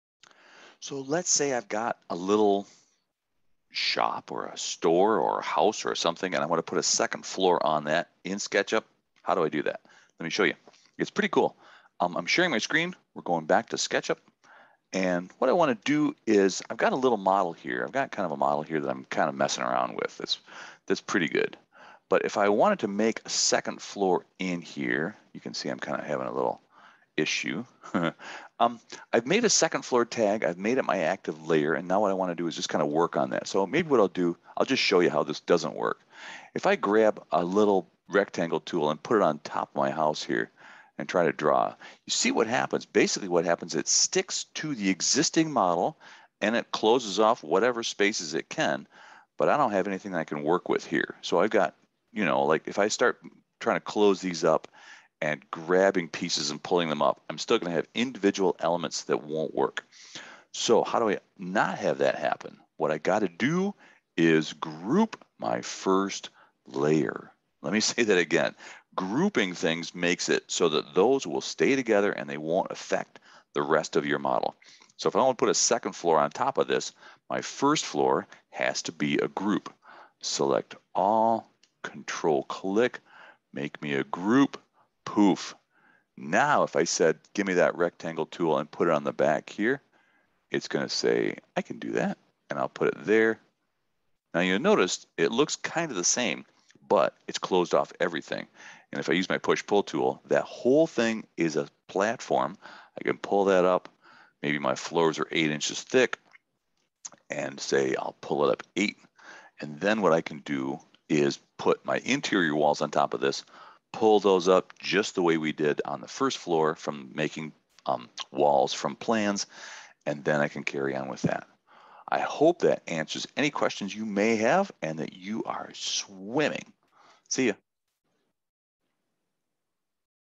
Second Floor
How to create a second floor on top of a one-storey building in Sketchup.